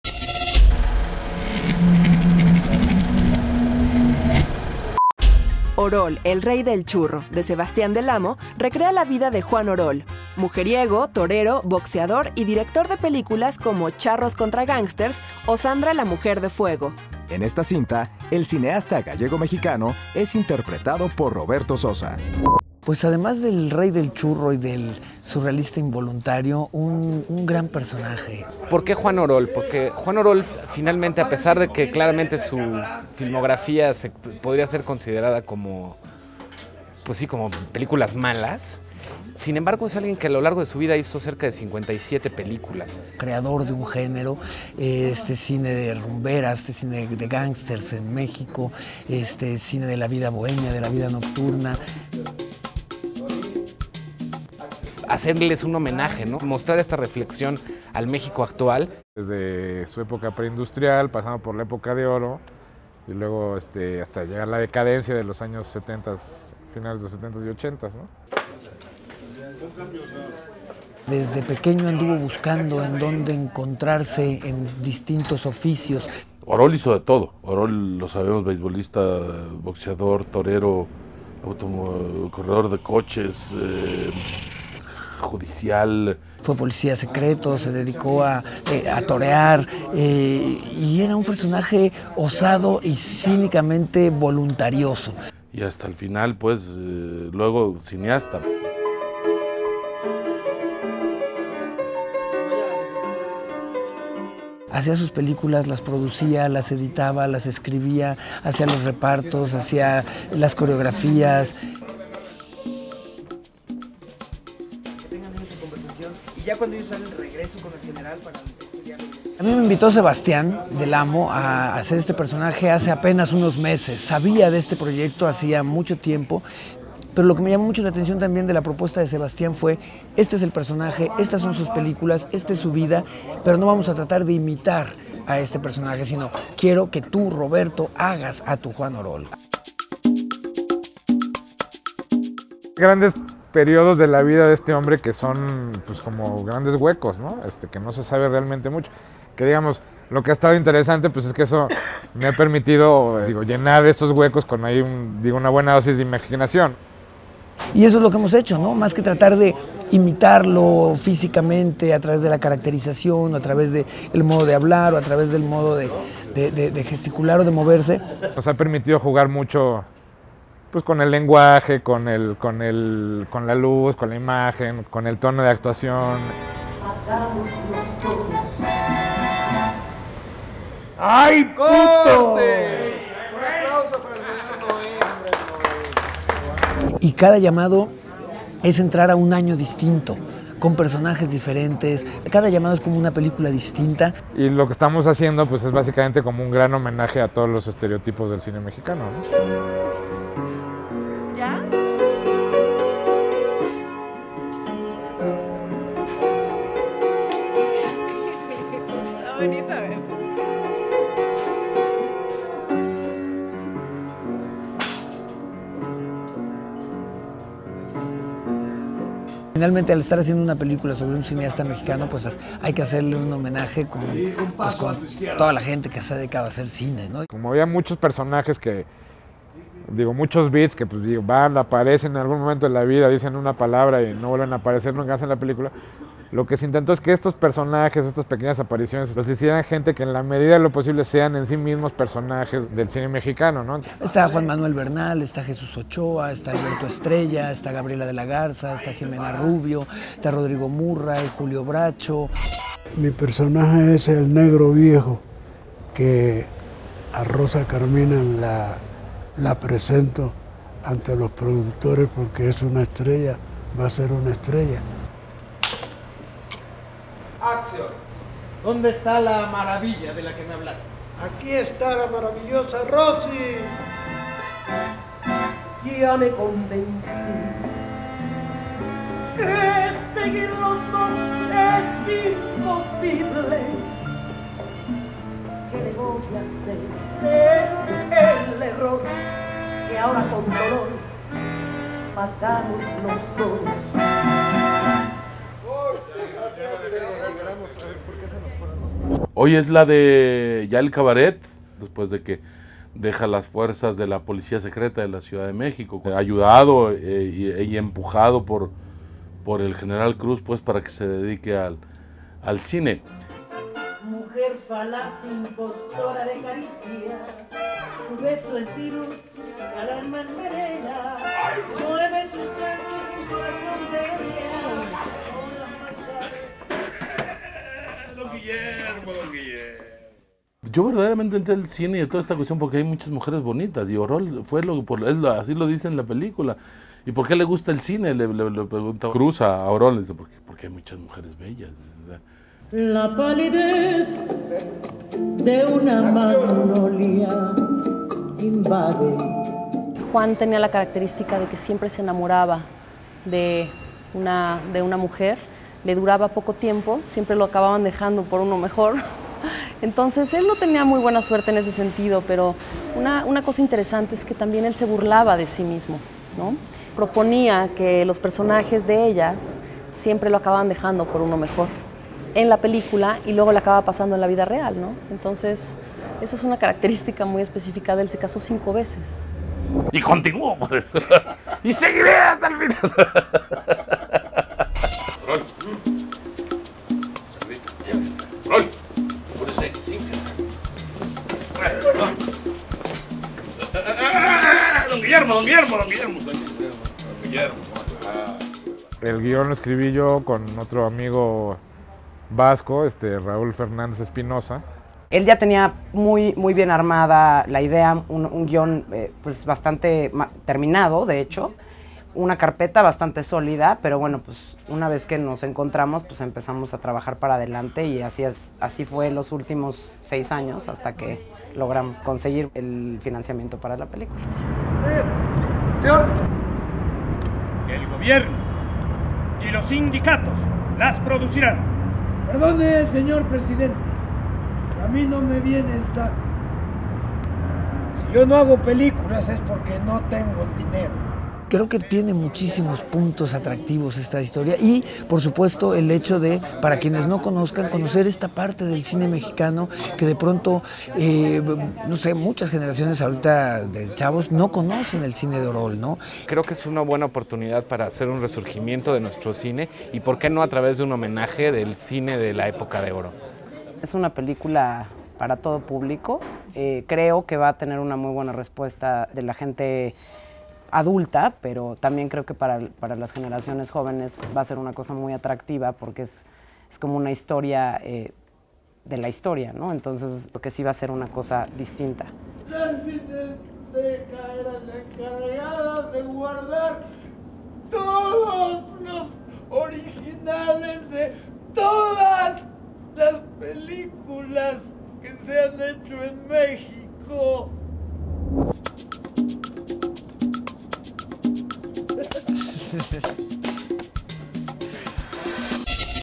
Rodaje, El fantástico mundo de Juan Orol
Entrevista a:

Sebastián del Amo, (Director)
Alejandro Velázquez, (Productor)
Roberto Sosa, (Interpreta a Juan Orol)
Jorge Zamora "Zamorita", (Actor)
Jesús Ochoa, (Actor)
Ximena González Rubio, (Interpreta a Rosa Carmina)
Carmen Ortega, (Productora)

Producida por Imcine, a través de Cine-SecuenciaS en la TV